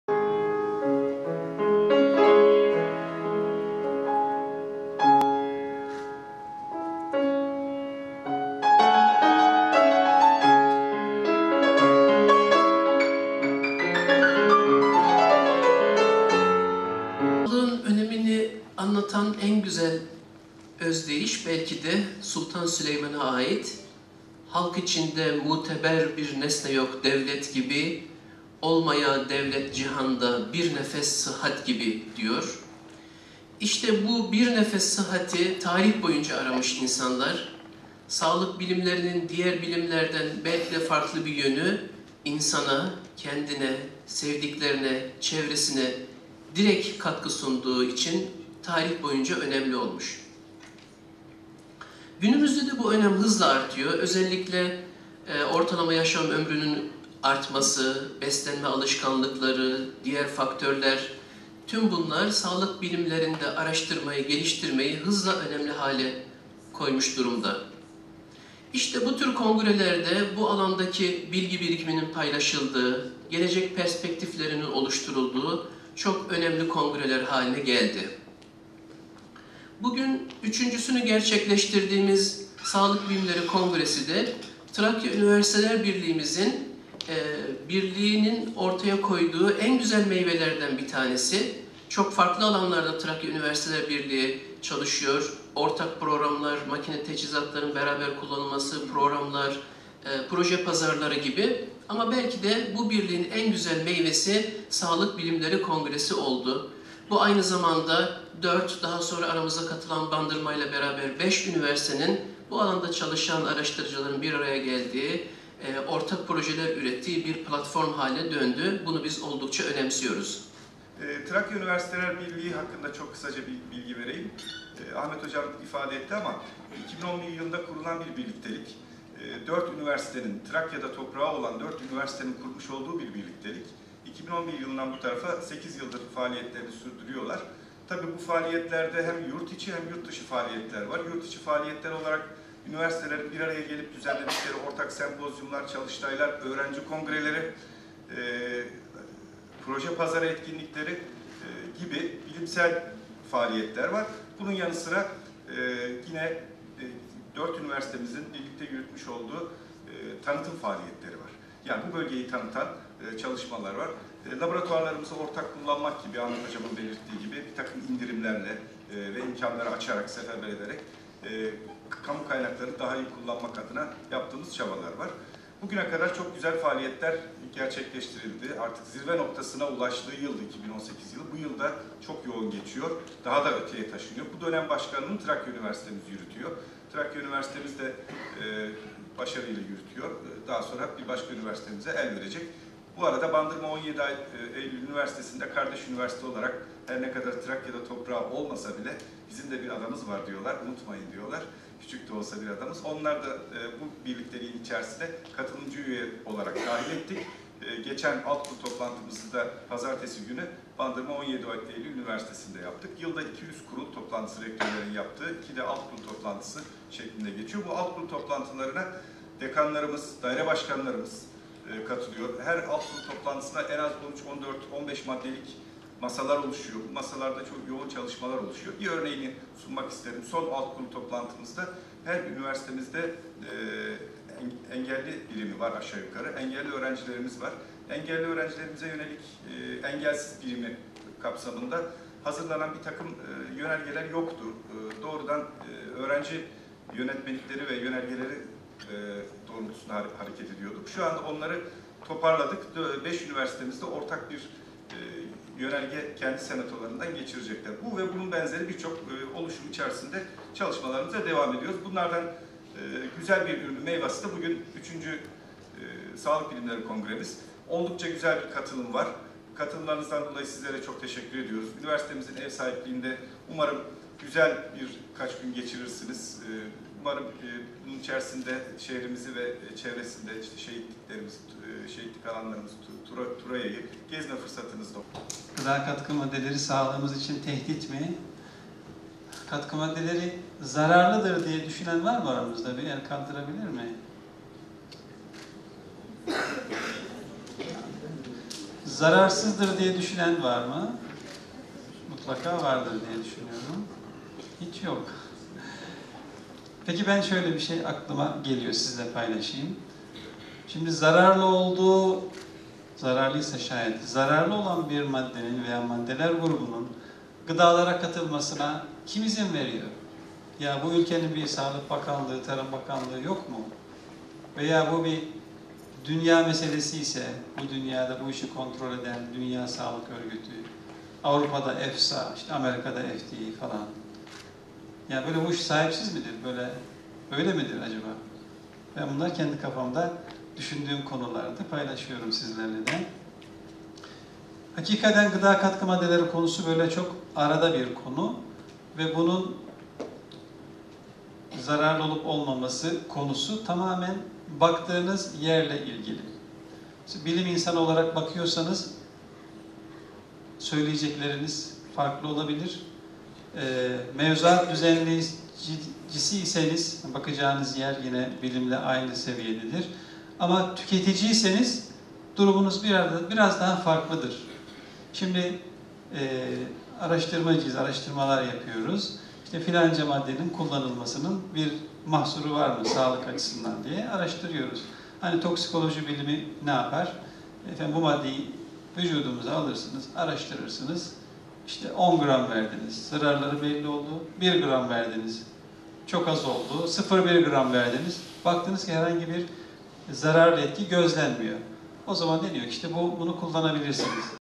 3. ULUSLARARASI SAĞLIK BİLİMLERİ KONGRESİ ÇOMÜ’DE GERÇEKLEŞTİ
Trakya Üniversiteler Birliği 3. Uluslararası Sağlık Bilimleri Kongresi Troya Kültür Merkezinde gerçekleşti.